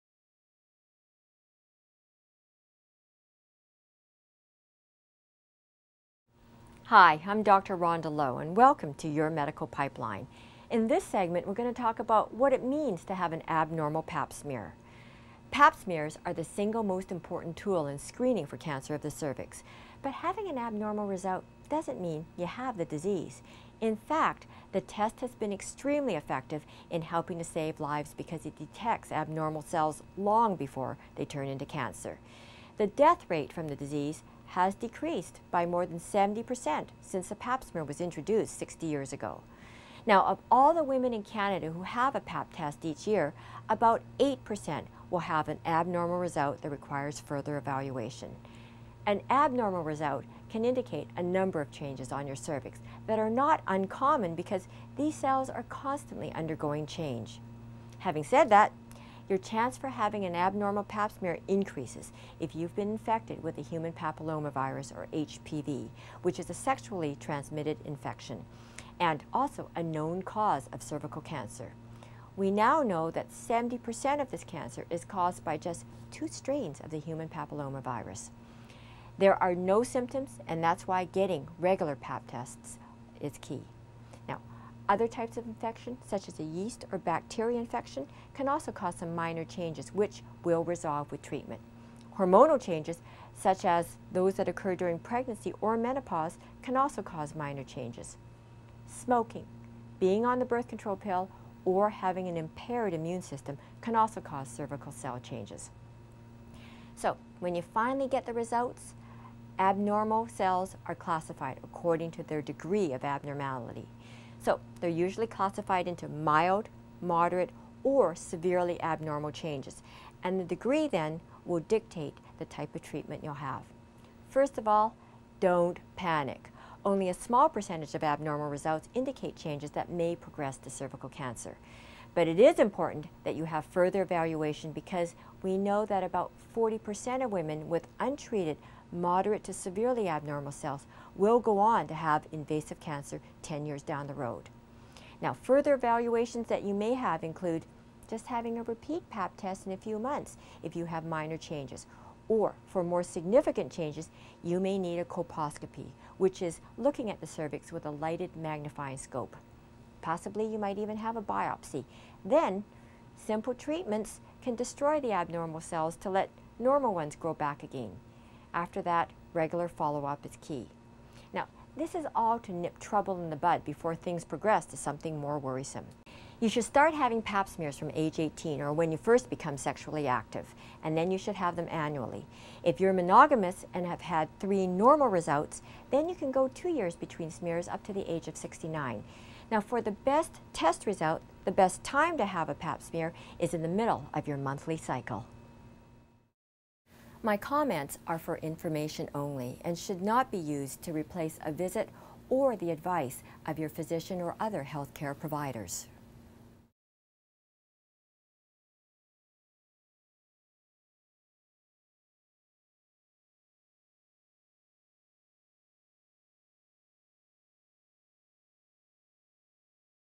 IF Online Women's Health - About Abnormal PAP Smears
The most important thing to do when you have an abnormal pap smear is NOT Panic, abnormal results don't necessarily mean cervical cancer. In this enlightening video you'll find out why pap smears are so important, the causes of abnormal results, and what to do when everything doesn't check out as normal.

This free video will help you:
Learn the ins and outs of pap smears
Gain knowledge for healthier living
Understand your body